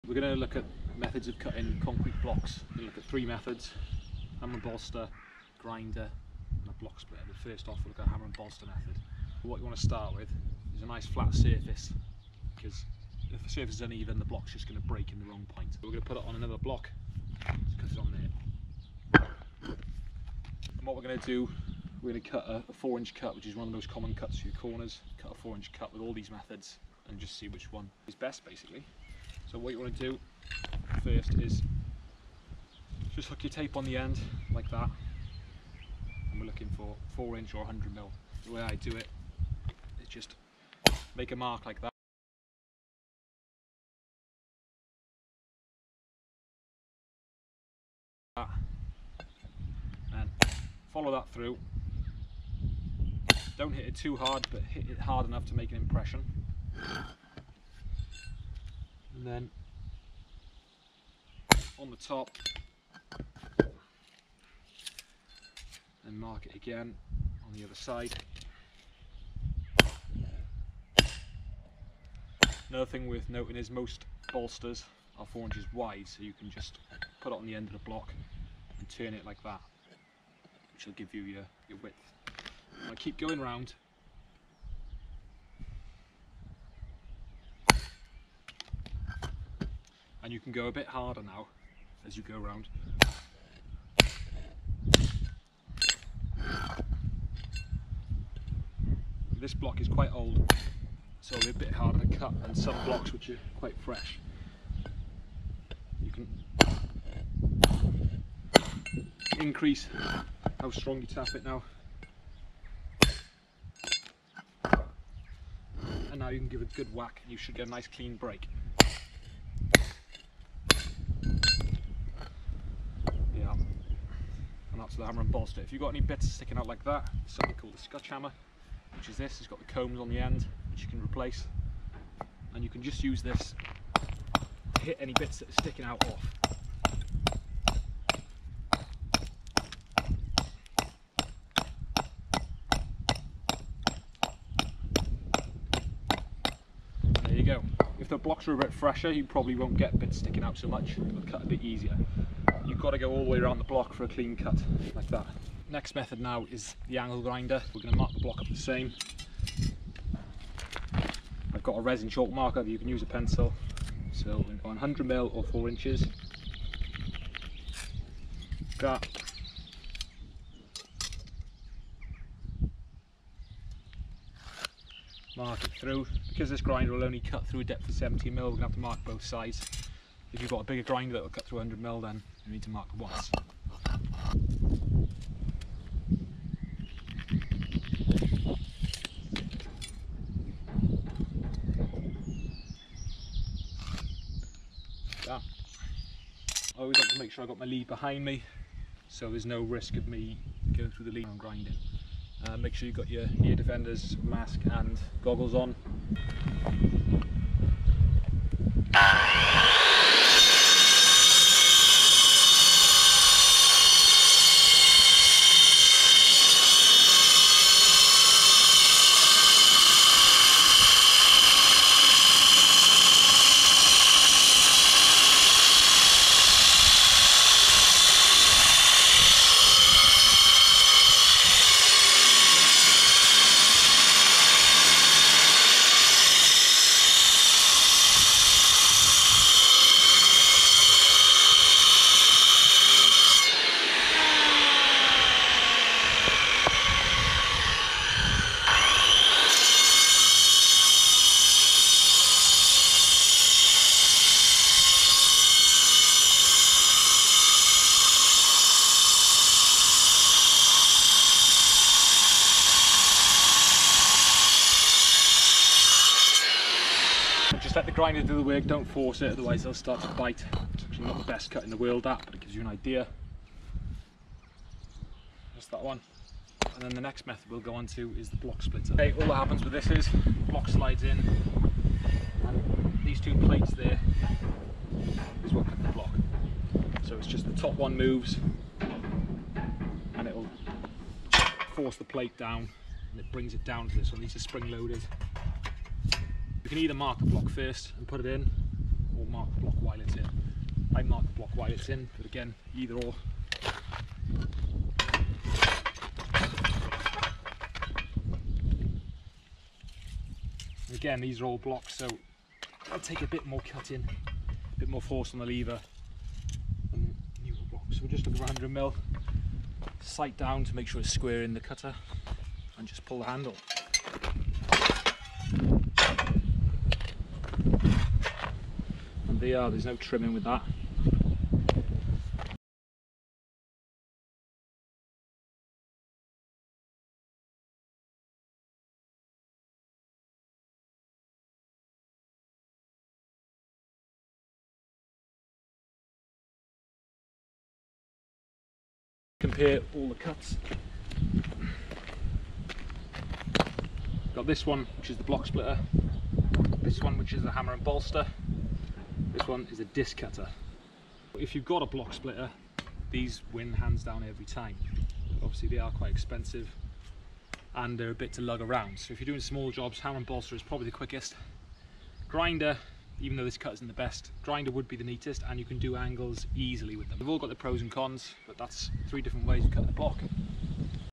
How to cut concrete blocks
3 different methods of how to cut concrete blocks using different tools. Number 3 is my favourite! Detailed video showing you how to cut concrete blocks or cinder blocks as they are sometimes called. Shows cutting concrete blocks using various tools! *Please ensure you wear eye protection with all of these methods*